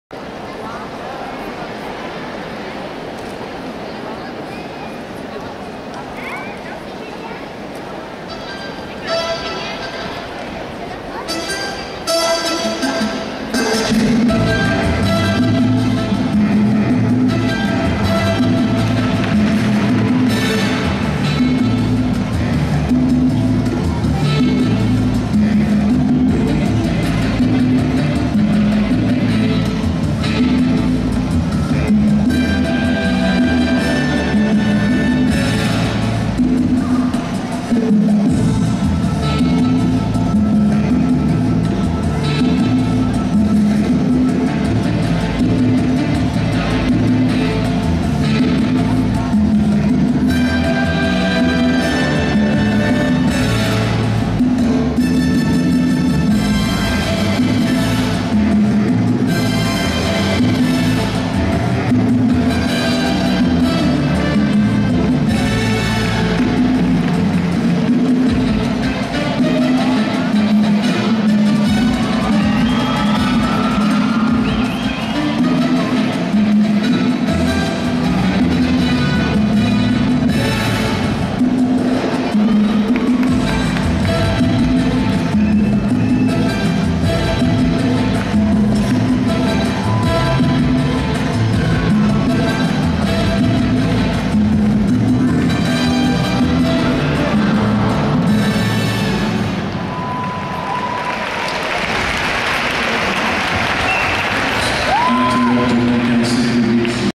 Yamilet PENA ABREU (DOM) floor - 2013 Antwerp worlds qualifs
Yamilet PEÑA ABREU (DOM)

'13 World championships in Antwerp - Qualifications

11.366 (D: 5.3 / E: 6.466 / P: 0.4)
Qualifs rank : 49th